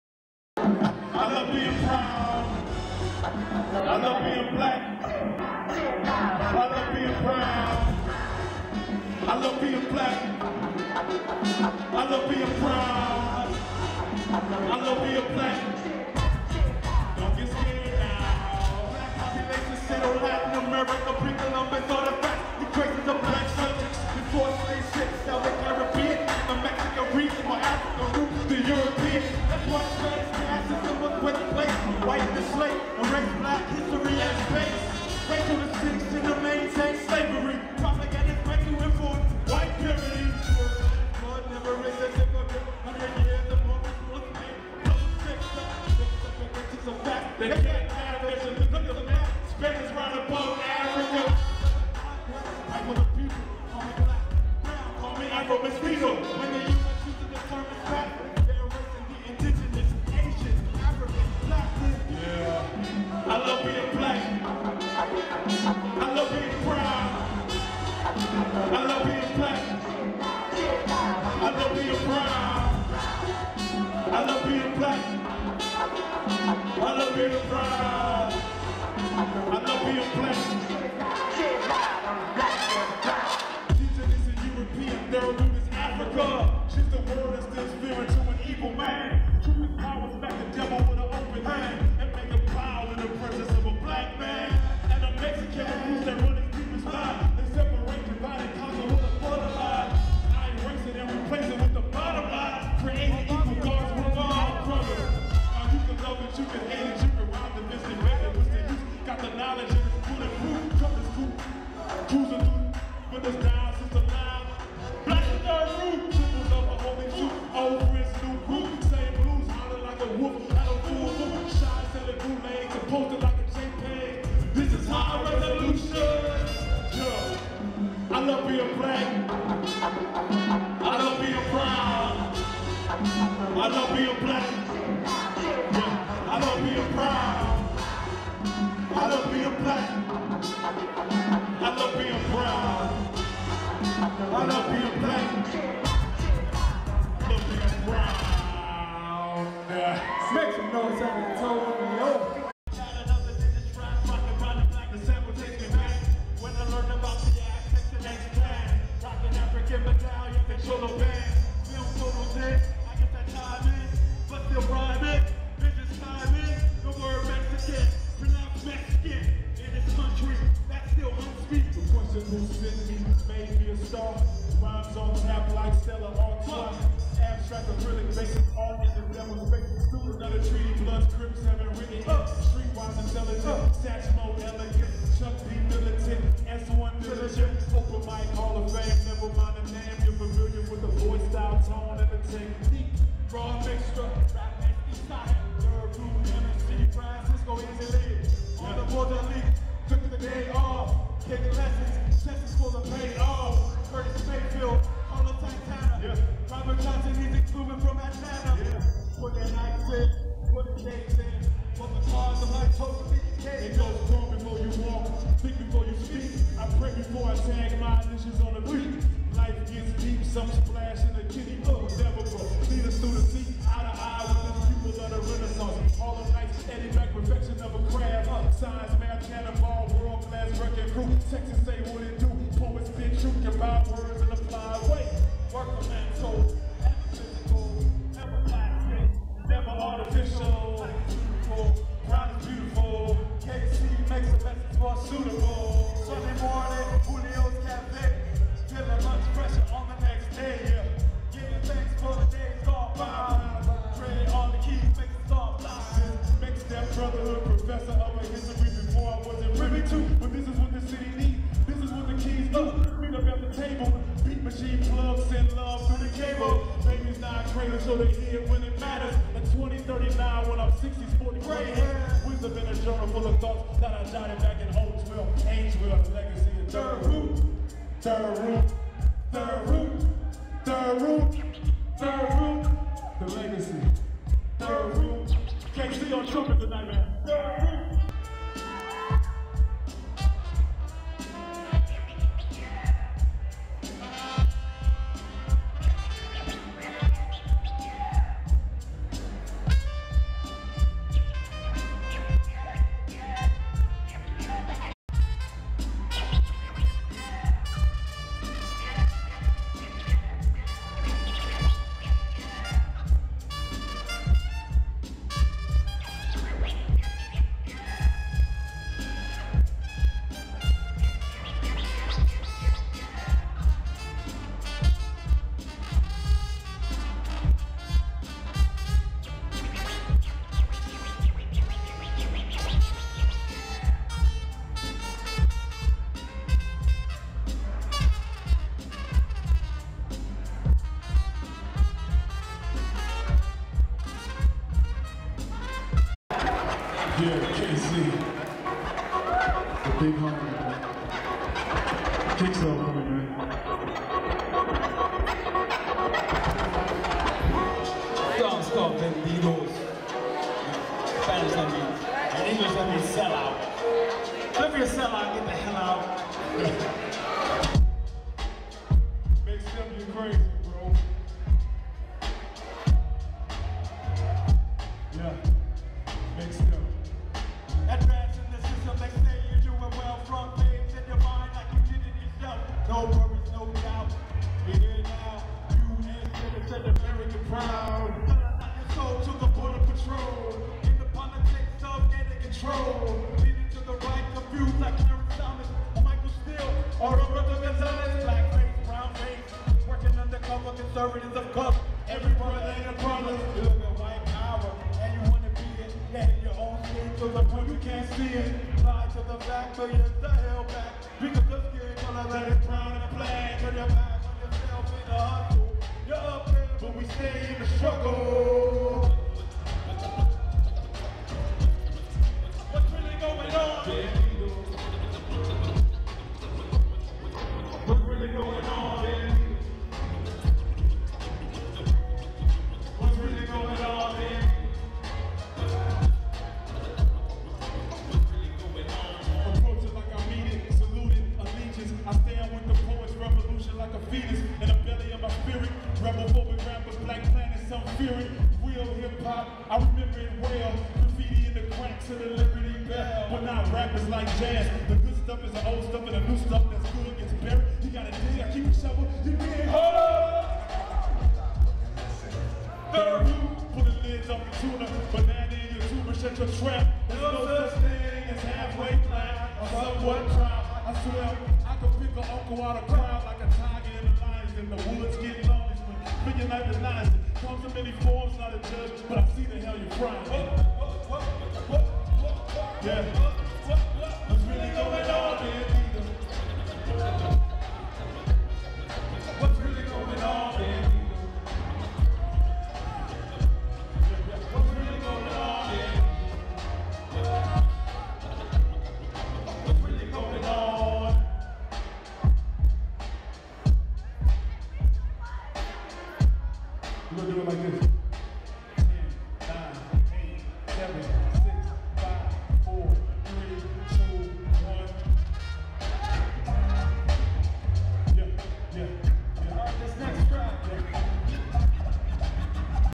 THIRD ROOT pt2 Live 2013 San Antonio Music Awards at the Aztec
THIRD ROOT Live 2013 San Antonio Music Awards at the Aztec Theatre 09/28/2013